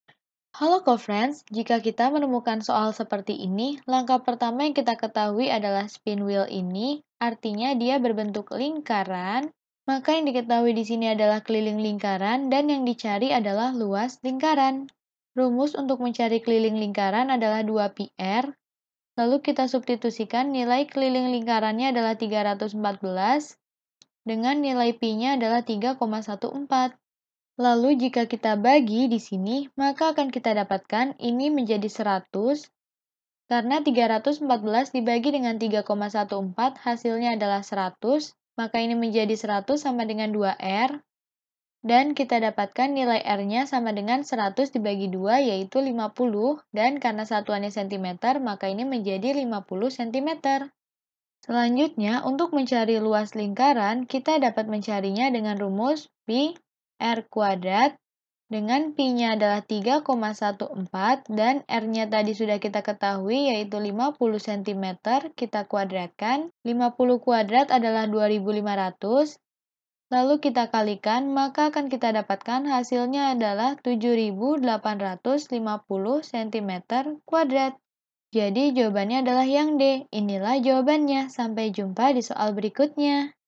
Iman memiliki mainan spin wheel dengan keliling 314 cm . Luas spin wheel tersebut adalah ... cm...
Iman memiliki mainan spin wheel dengan keliling  314 cm . Luas spin wheel tersebut adalah ...  cm^2

Topik atau Materi: Penerapan Keliling dan Luas Lingkaran Pada Soal Cerita - LINGKARAN - GEOMETRI - Matematika Kelas 8 SMP -

00:00 : Bedah Soal
01:00 : Konsep, rumus dan pengertian Penerapan Keliling dan Luas Lingkaran Pada Soal Cerita
01:18 : Langkah penyelesaian soal
00:23 : Jawaban akhir
00:53 : Penutup